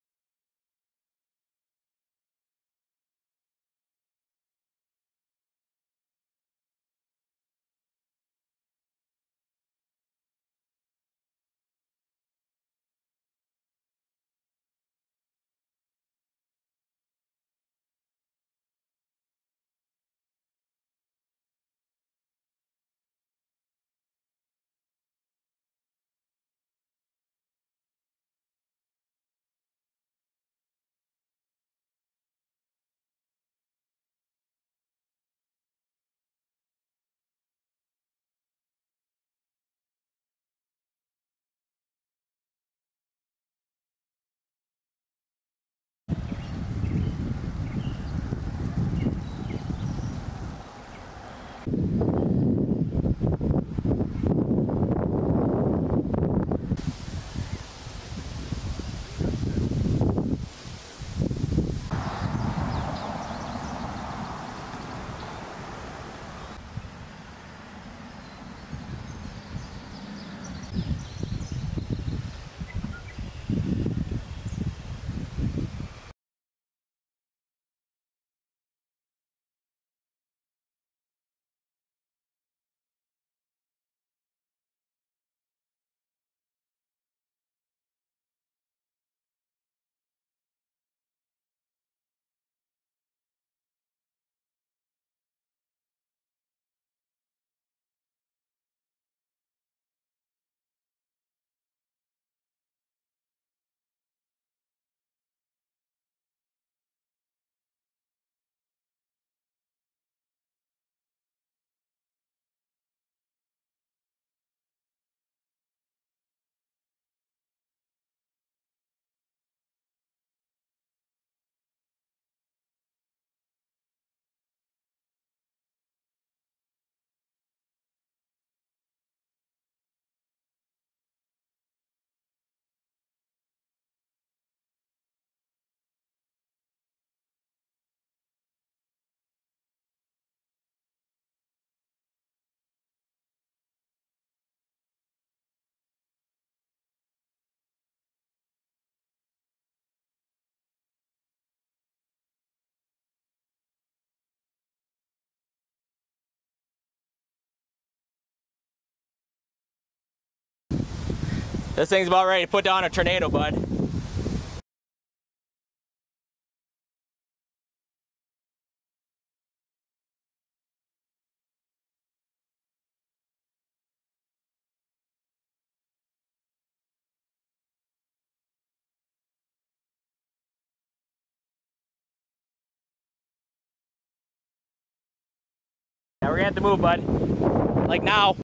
06-15-2022 Hillsboro, WI - Tornado Damage
Strong storms moved through Wisconsin Wednesday afternoon. This line of storms spawned a few tornado warnings, some with confirmed tornadoes. A tornado near Hillsboro, WI caused major damage. Fortunately, no injuries were reported.